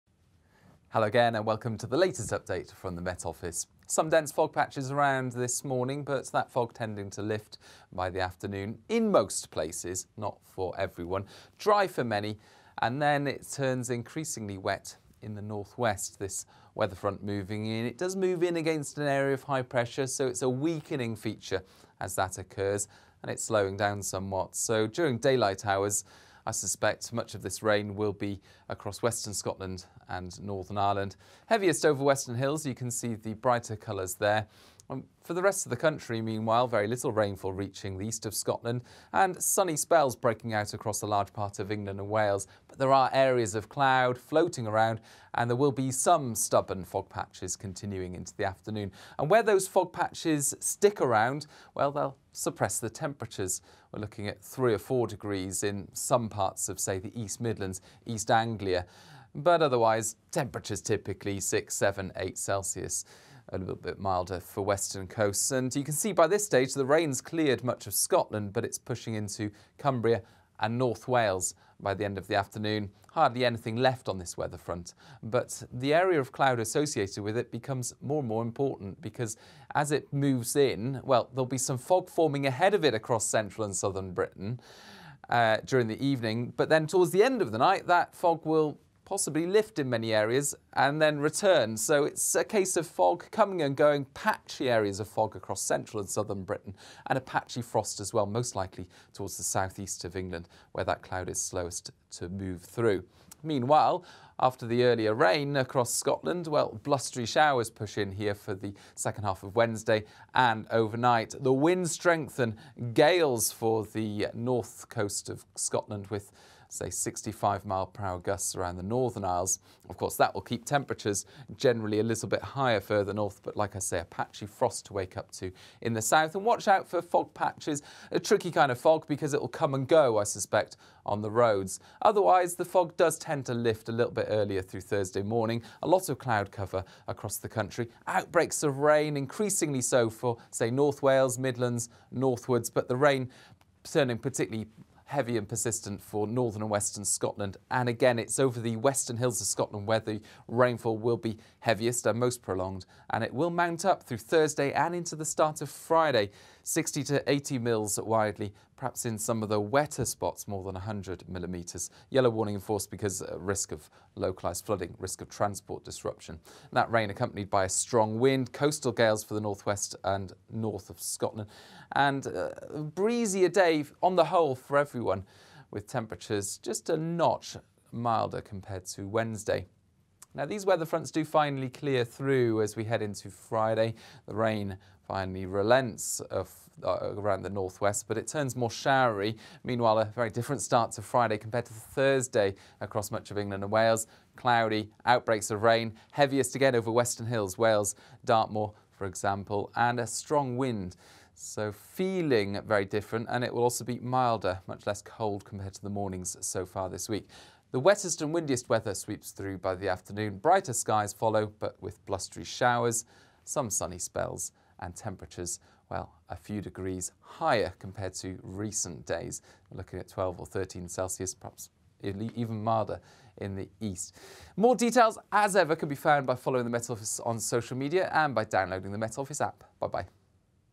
Wednesday mid-morning forecast 04/12/19
National weather forecast presented by Aidan McGivern.